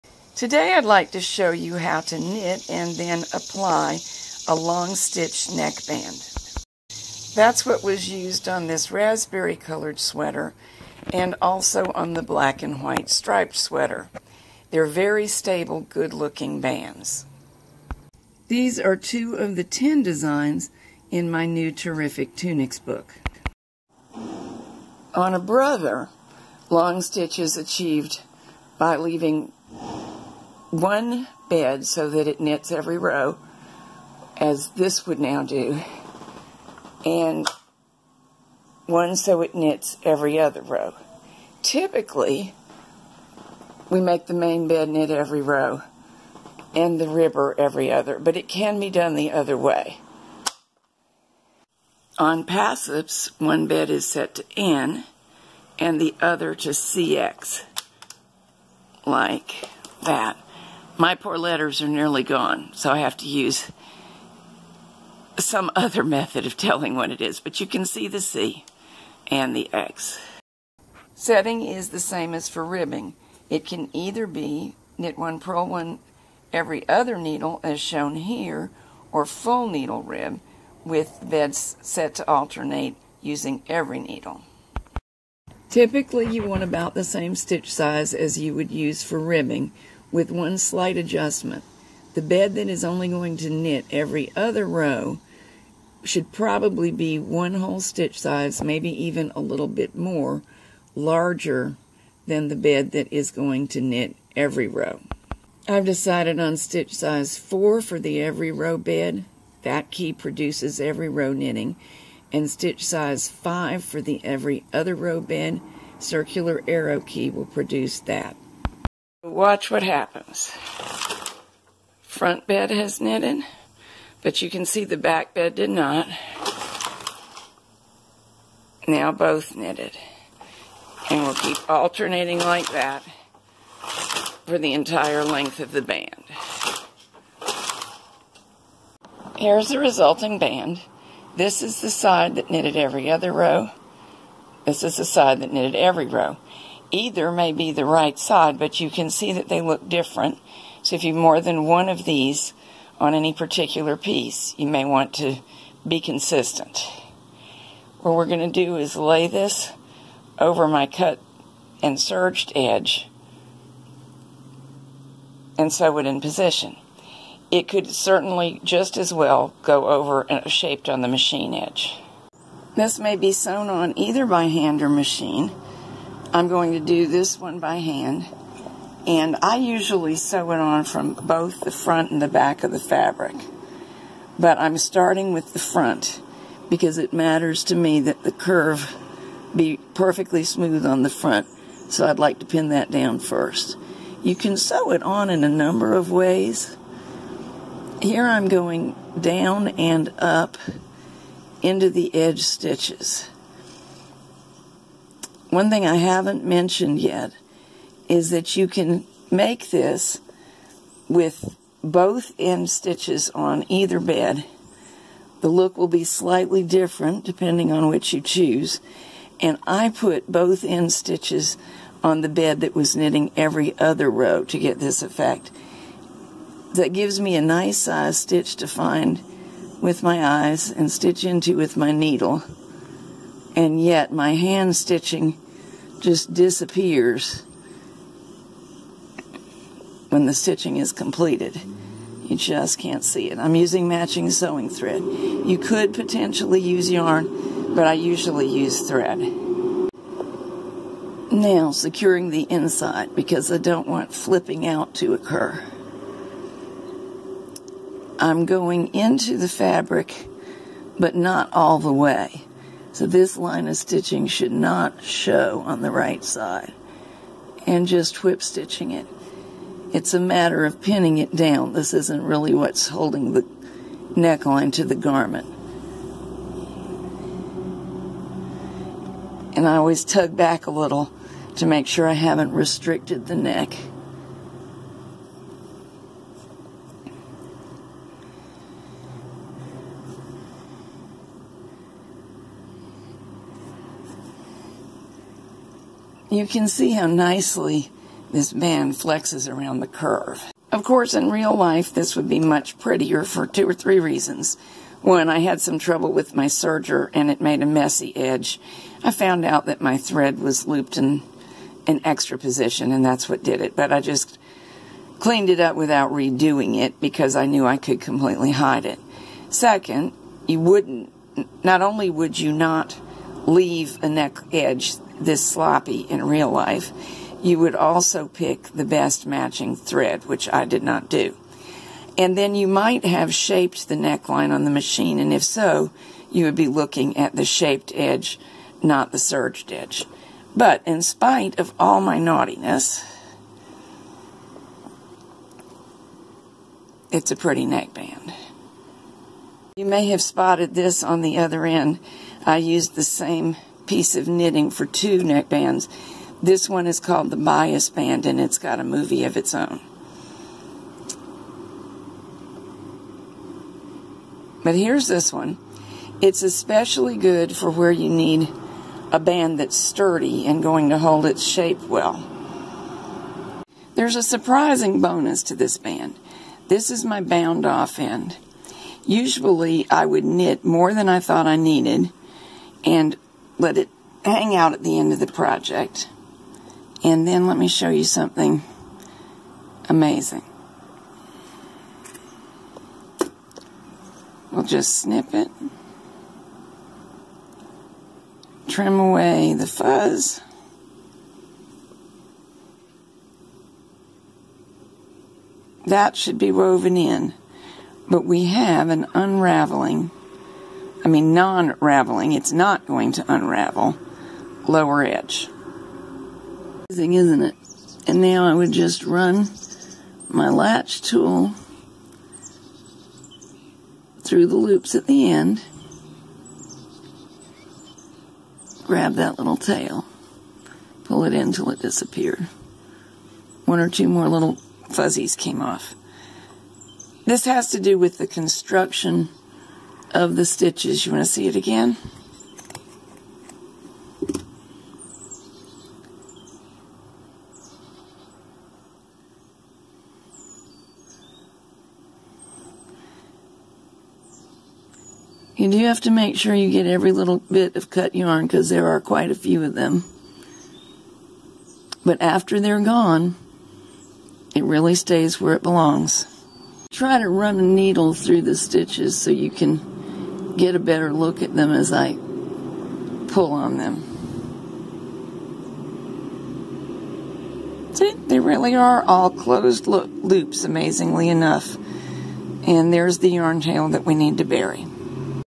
Long Stitch Neckband
This is an exceptionally stable, easy to make neckband. it is sewn onto the shaped neckline. shaping may be done on the knitting machine or using cut and sew techniques. the band requires 2 beds. the video shows how to knit the band as well as how to attach it. this kind of neckband is used in 2 of the designs in my new book: TERRIFIC TUNICS available in my Ravelry store.